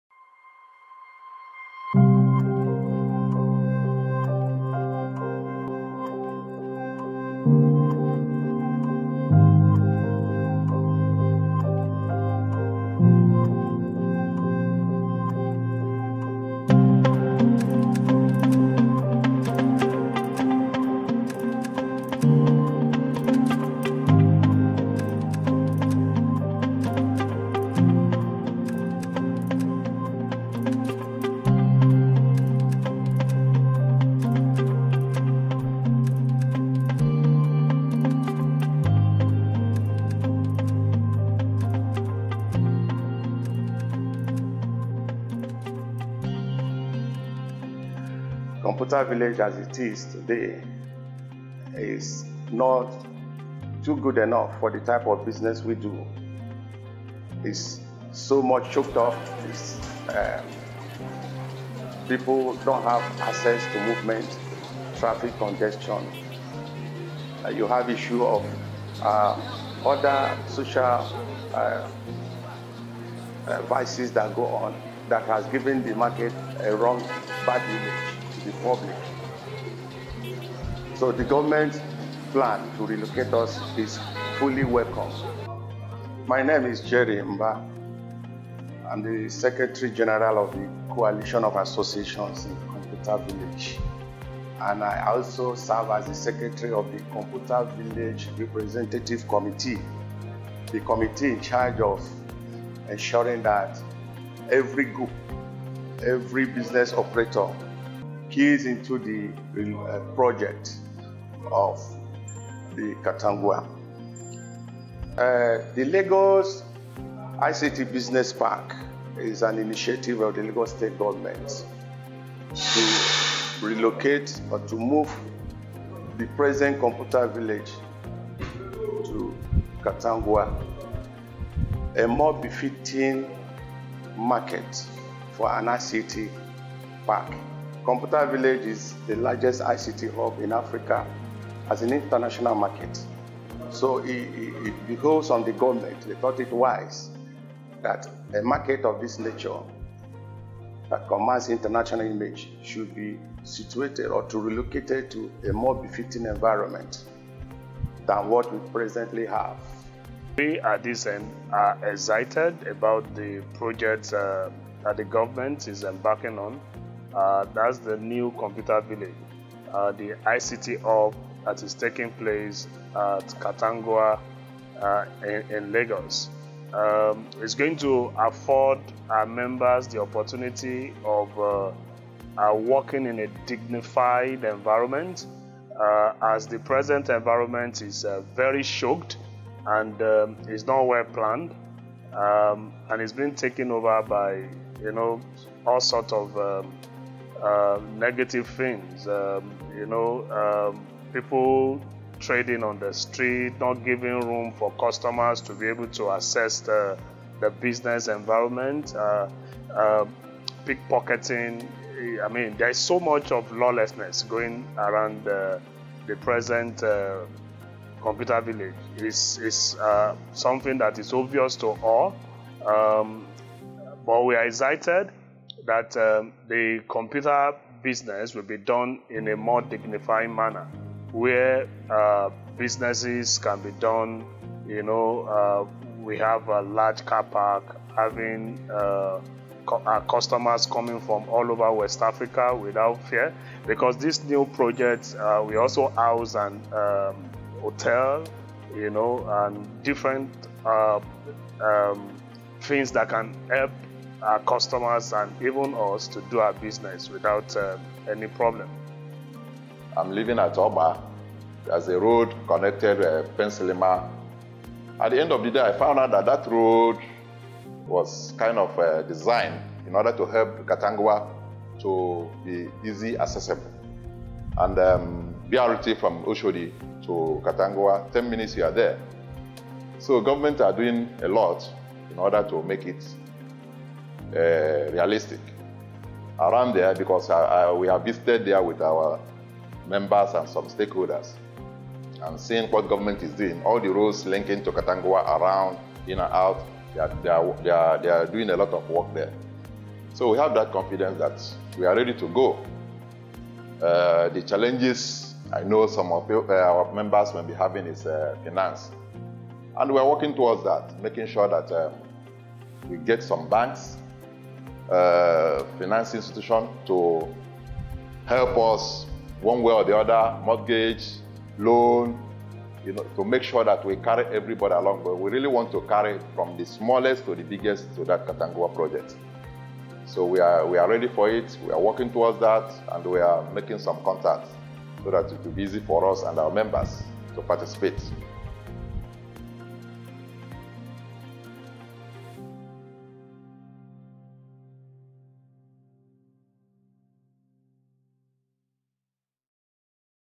Katangowa ICT Park Documentary
A short documentary about the Katangowa ICT park, currently under development. Key stakeholders also give their takes on the transition from Old computer village to the future.